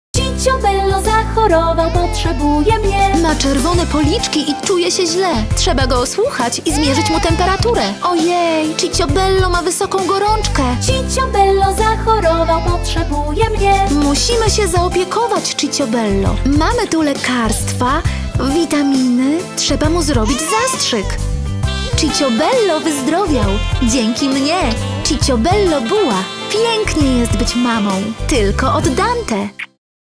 Lalka Cicciobello Bua Przeziębiony | Reklama
Cicciobello Bua to wyjątkowa lalka, dzięki której pokochasz bycie mamą!
Bua to przeziębiony chłopiec, któremu musisz pomóc wrócić do zdrowia. Wykorzystaj lekarstwa, witaminy, osłuchaj go i zmierz mu temperaturę. O nie! Ma zaczerwienione policzki! Trzeba podać mu zastrzyk.

Cicciobello Bua to kompletny zestaw zawierający lalkę funkcyjną i akcesoria do jej zabawy, które stanowią świetny pomysł na prezent.